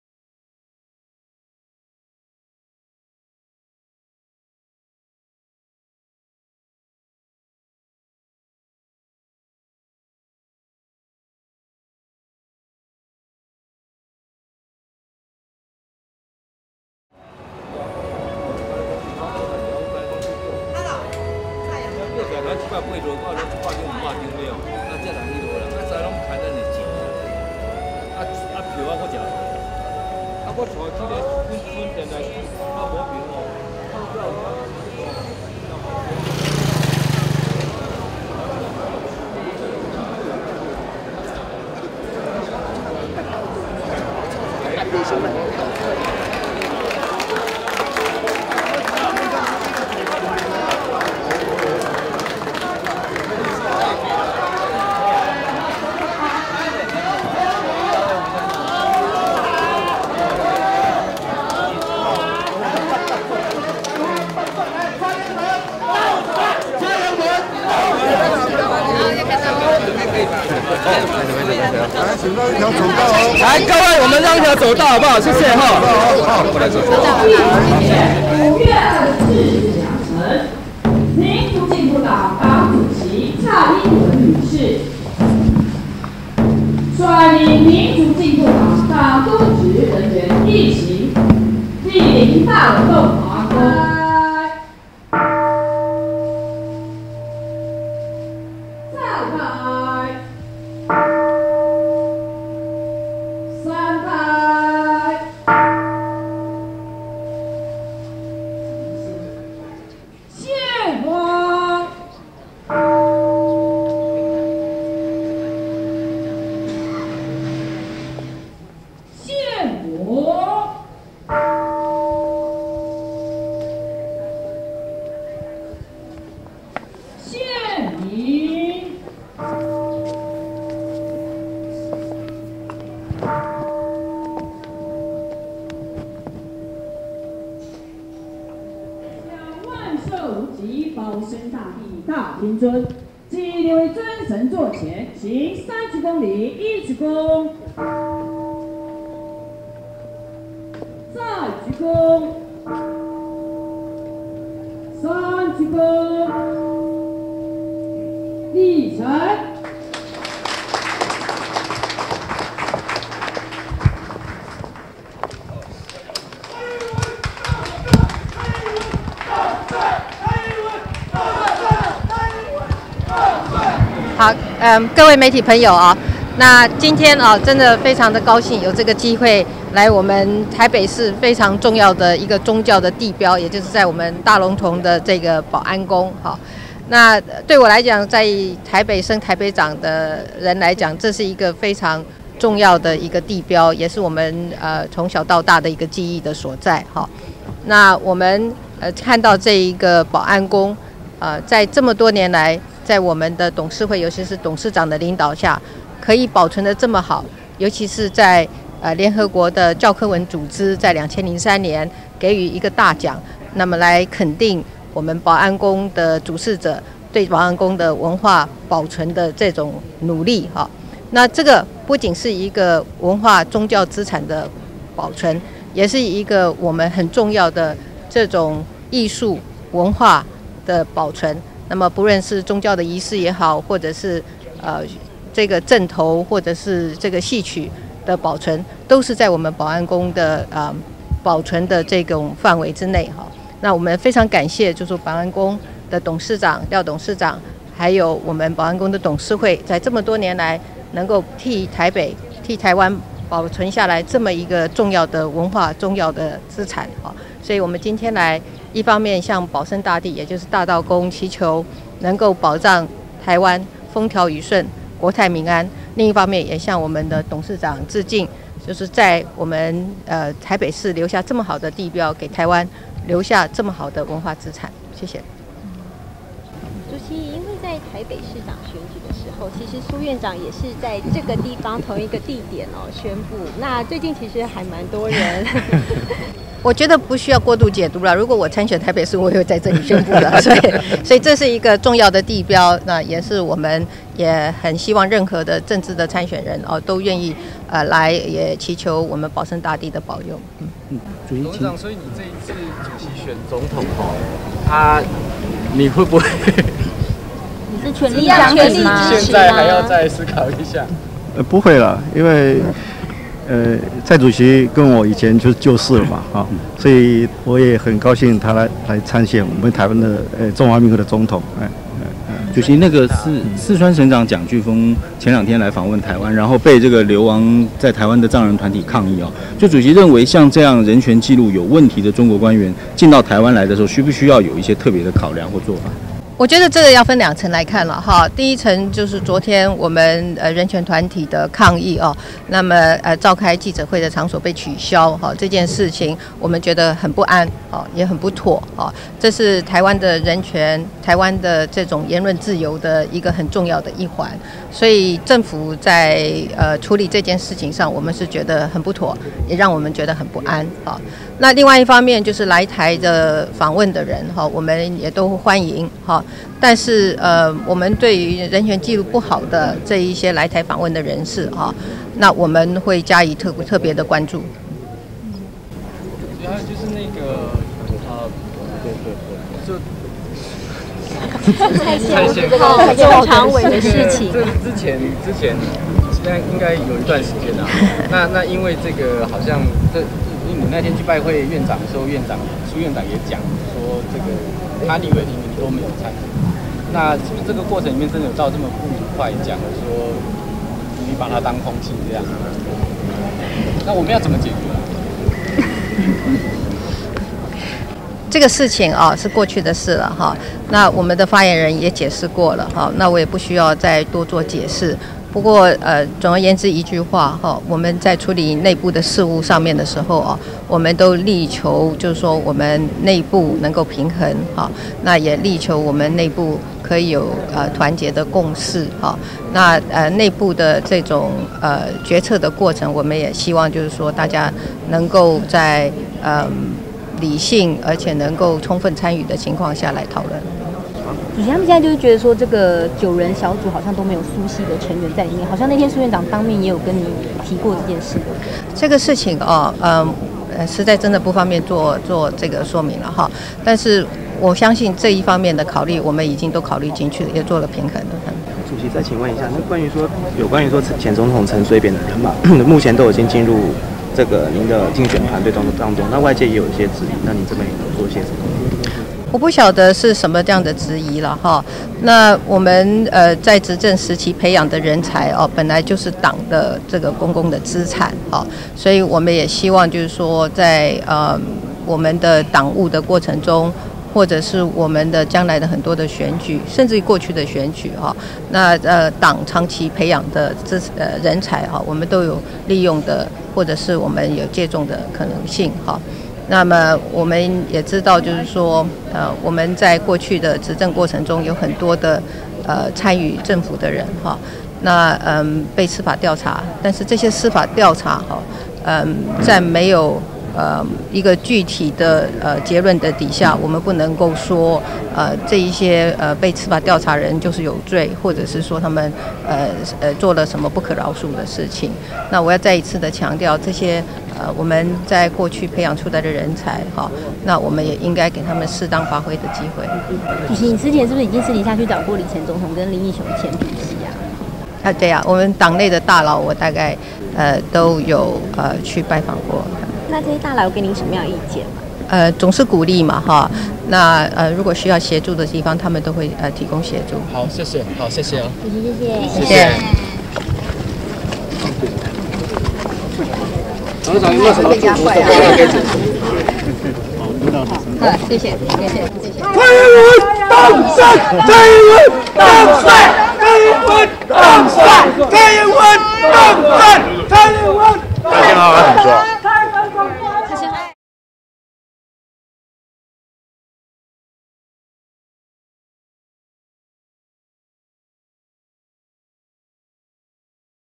蔡英文主席保安宮祈福參拜
民進黨主席蔡英文今（5/24）日上午前往保安宮祈福參拜，蔡英文表示很高興可以來到這個台北市重要的地標『保安宮』，對他這個在台北生台北長的人來說，這是一個非常重要的地標，也是從小到大的記憶所在。

蔡英文說，保安宮這麼多年來在董事會及董事長的領導下保存得這麼好，聯合國的教科文組織在 2003年也曾給予大獎，來肯定保安宮的主事者對保安宮文化保存的努力。這不僅是一個文化宗教資產的保存，也是很重要的藝術文化的保存，不論是宗教儀式、陣頭、戲曲都是保安宮文化保存的範圍，蔡英文表示非常感謝保安宮廖董事長及董事會這麼多年來替台灣保存這麼重要的文化資產，因此她今天來不僅是來向保生大帝祈求保要台灣風調雨順、國泰民安，另一方面也是來向董事長及董事會致敬，感謝他們對文化資產的保存。保安宮廖董事長被問到是否支持蔡主席時他表示，他跟蔡主席是舊識，很高興看到蔡主席參選 2012總統。

對於台灣圖博之友會、台灣關懷中國人權聯盟等團體昨日在圓山飯店要召開記者會，抗議來台的四川省長迫害人權，卻被圓山飯店臨時取消因而造成衝突事件，蔡英文表示，這必須要分為二層來看，首先是人權團體的抗議，召開記者會的場所被取消這令人感到不安，也很不妥，這是台灣的人權、台灣的言論自由很重要的一環，政府這樣的處理手法令人感到不安與不妥。另外，對於任何來台訪問的人都應該歡迎，但對於人權記錄不好的來台人士則應該要加以關注。

而日前拜會蘇貞昌時他提到立委提名過程都沒參與，是否是把蘇貞昌當空氣？蔡英文表示，這事情是過去的事了，發言人也解釋過了她不需要再多做解釋，不過民進黨在處理內部事務上都力求內部能夠平衡，也力求內部能團結有共識，內部的決策過程也希望大家都能夠在理性及充分參與的情況下來討論。對於蘇貞昌認為九人小組裡都沒有蘇系的成員在內，蔡英文說這方面不方便做說明，但她相信這方面的考慮都在他們的考慮範圍，也做了平衡。

而外界質疑陳前總統的人馬都進入了她的競選團隊，蔡英文說她不知道這是什麼樣的質疑，在執政時期培養的人才本來就是黨的公共資產，也希望在黨務及選舉都能夠借重這些黨長期培養的資產。而過去執政過程中很多參與政府的人被司法調查，但這些司法調查在沒有具體的結論底下不能說這些人就是有罪，或者說他們做了不可饒恕的事情，蔡英文也再次強調這些黨過去培養的人才也應該給他們適當發揮的機會。

蔡英文私底下也拜會過李登輝及林義雄等大老，蔡英文說這些前輩給她很多鼓勵，也表示需要協助的話他們會盡全力協助。